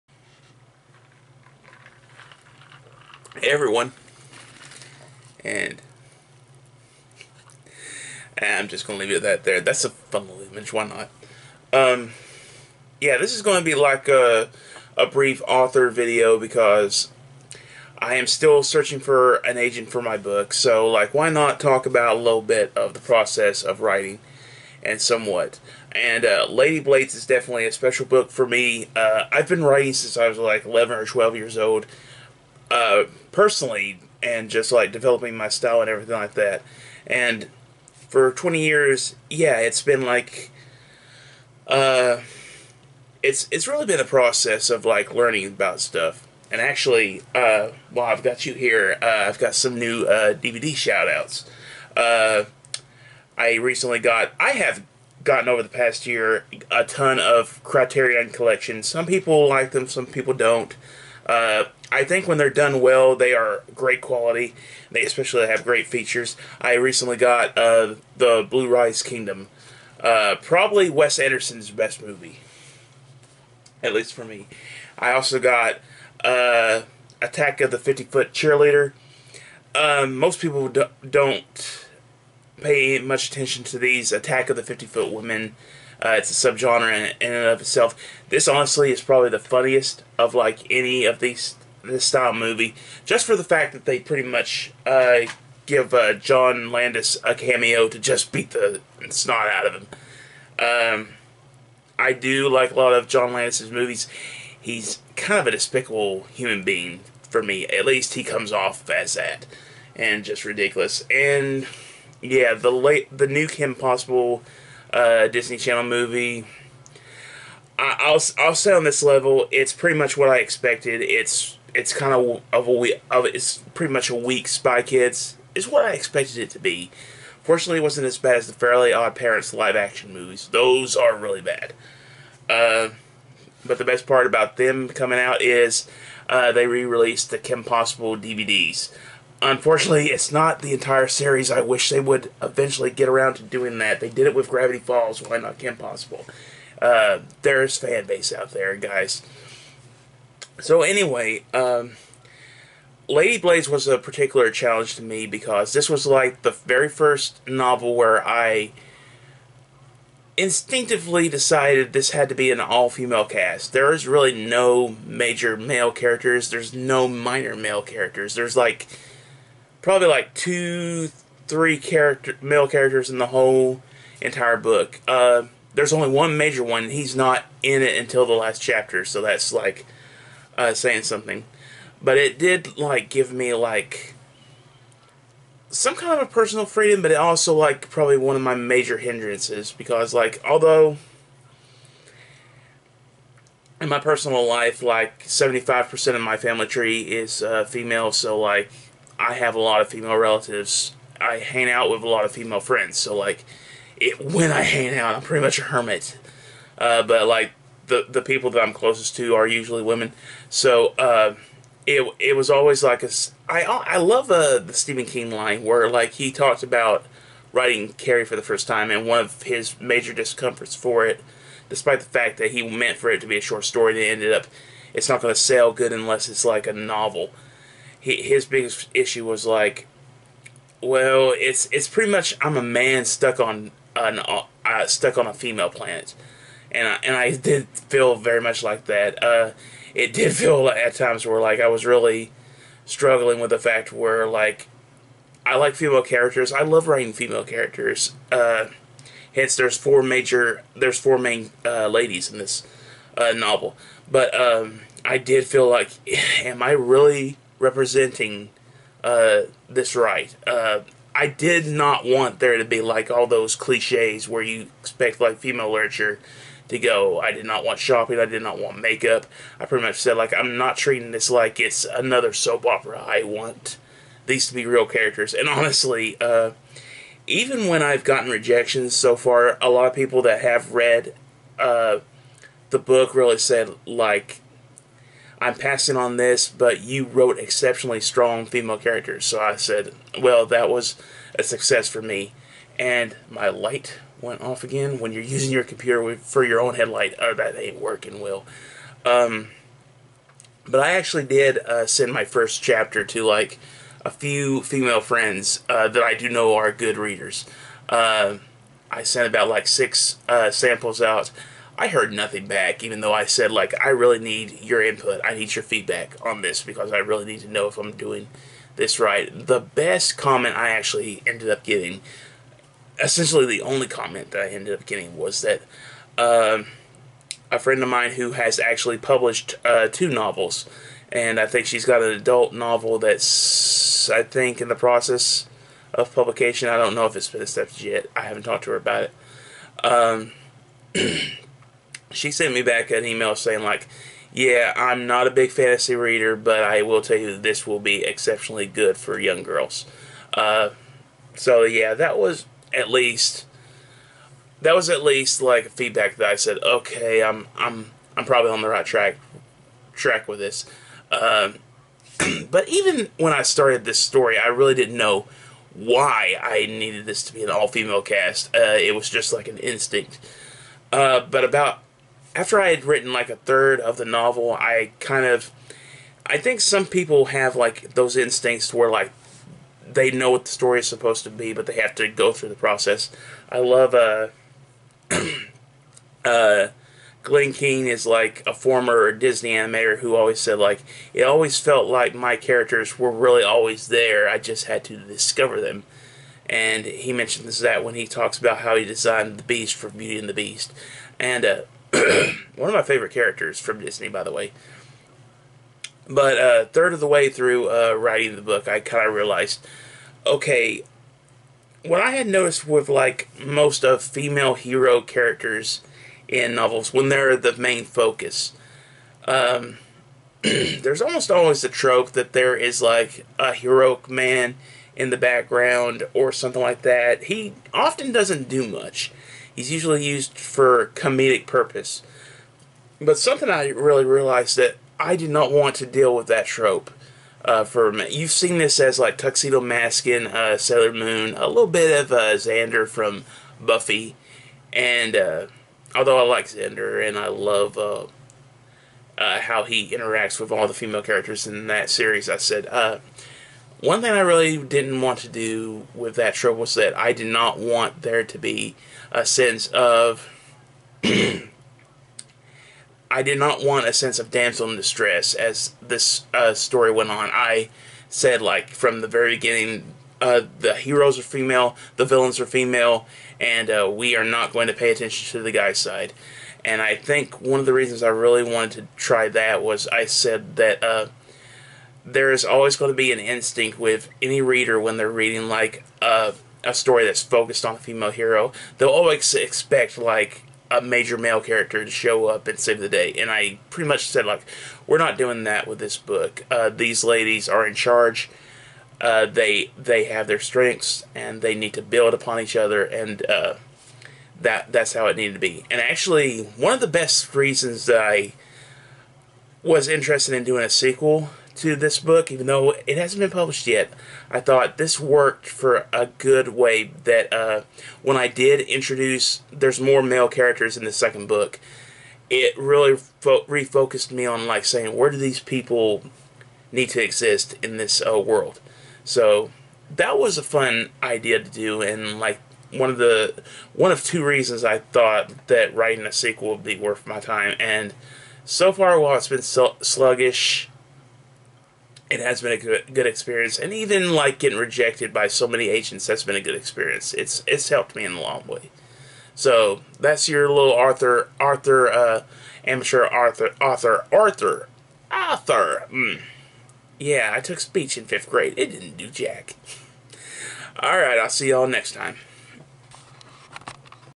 AMATEUR AUTHOR VLOGS #1---"A Trip Through Planet Female"
While LEGENDS OF THE LADYBLADES wasn't my first try at Fantasy, it's pretty unique in that I challenged myself with using a largely female-centric cast of characters (there's literally no leading or supporting male characters, and the only significant one doesn't show up until the final chapter.

So, here's me talking about dealing with this new challenge with all its reveals and reflections of my personality (and some quick DVD reviews too)